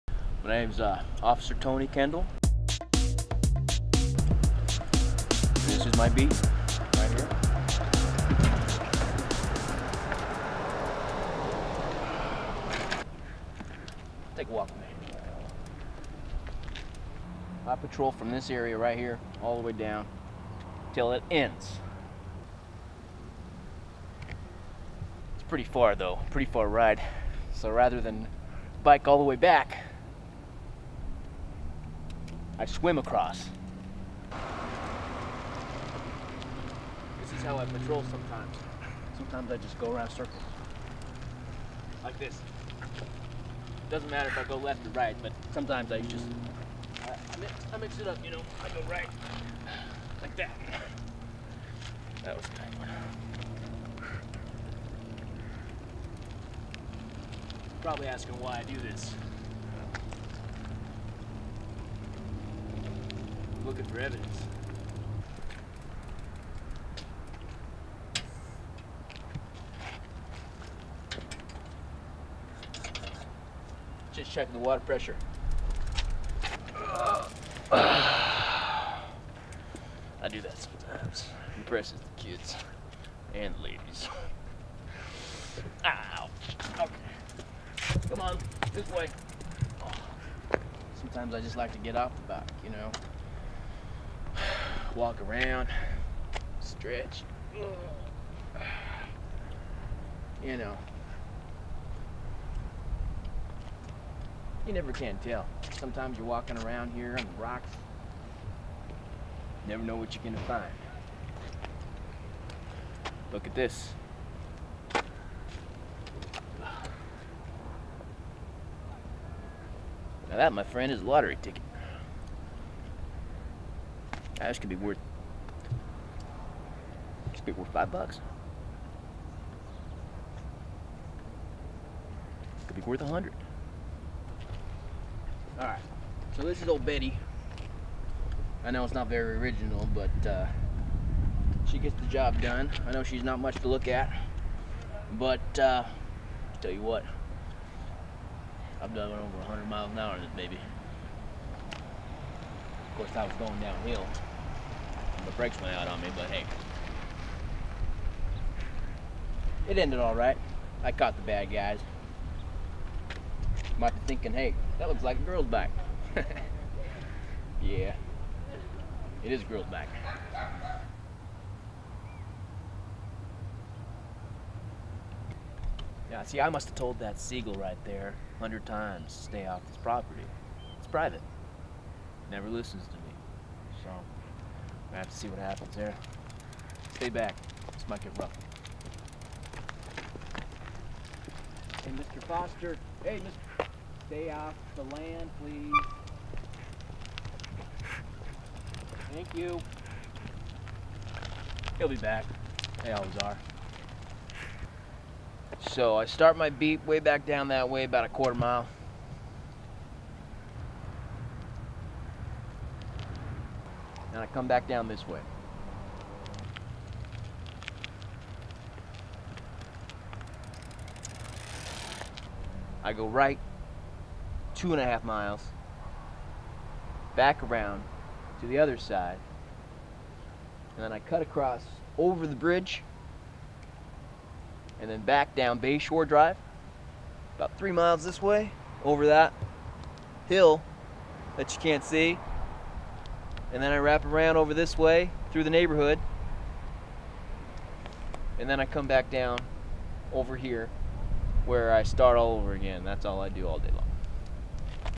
Tony Kendall Part 1
Follow Tony Kendall as he patrols the mean streets of Belmont Shore.